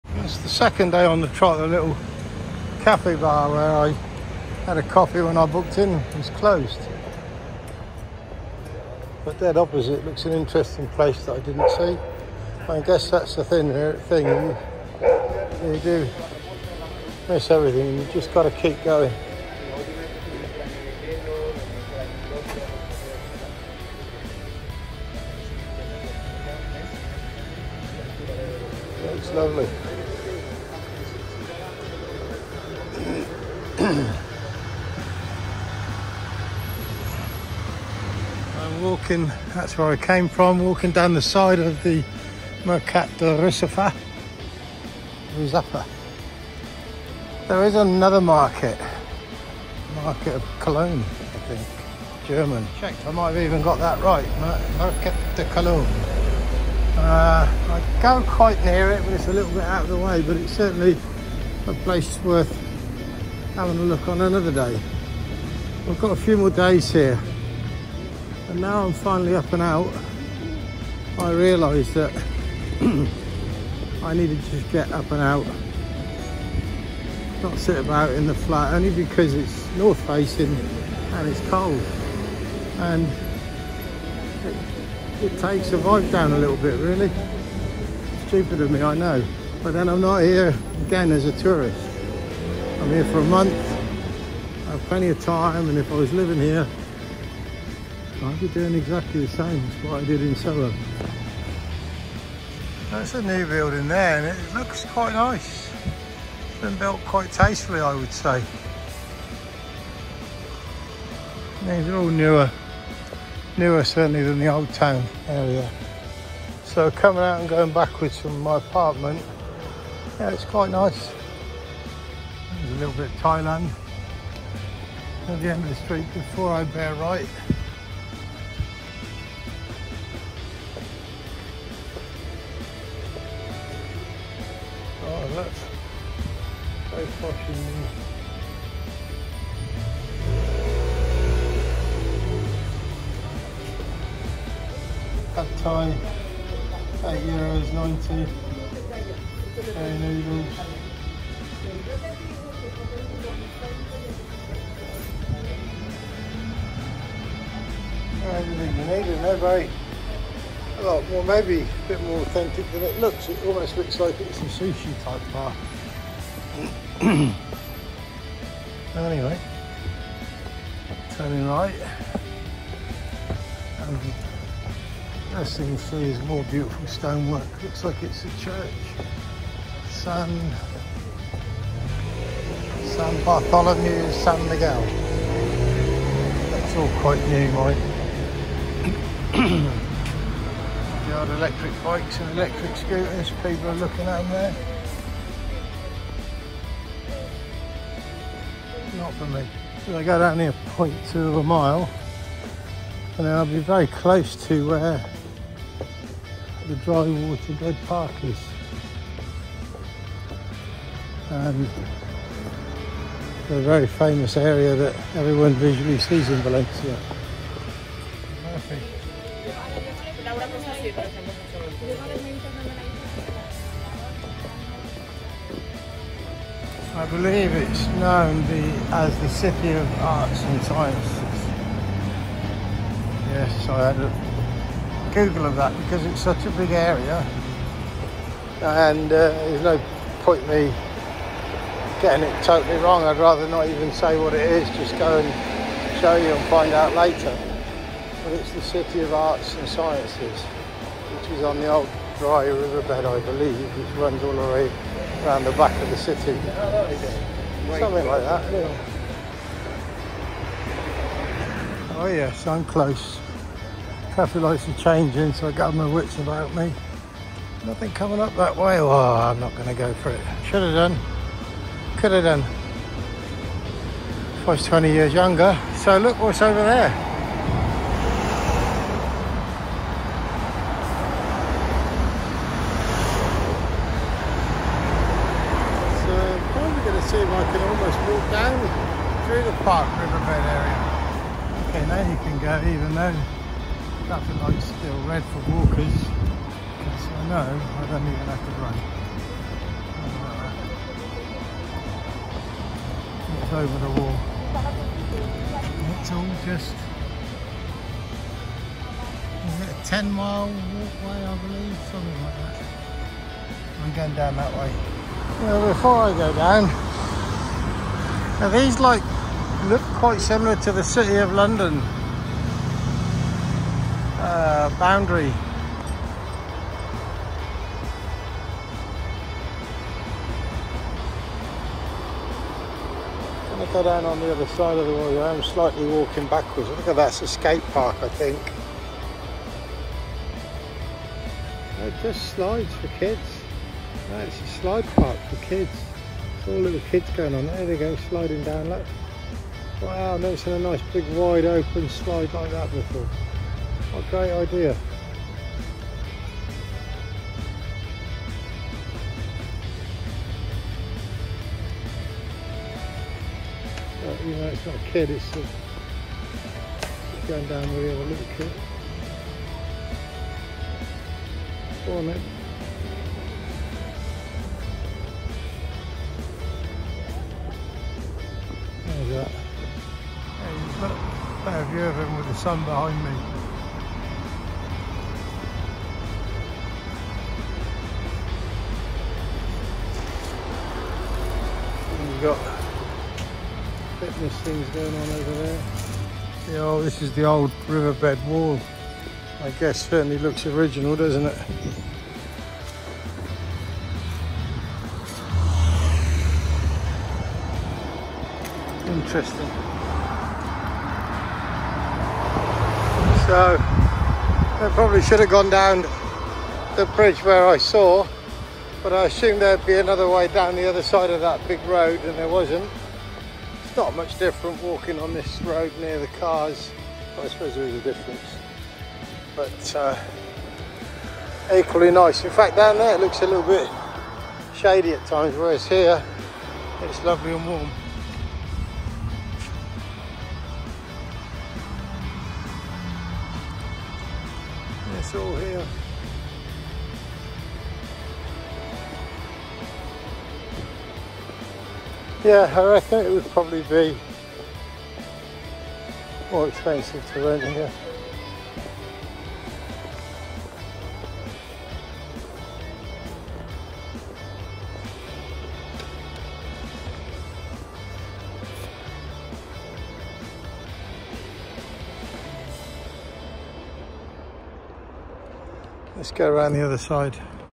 Walk to The City of Arts & Science Part 1. 10-01-23
Walking to the Ciudad de las Artes y las Ciencias along the old river bed. Part 2 to follow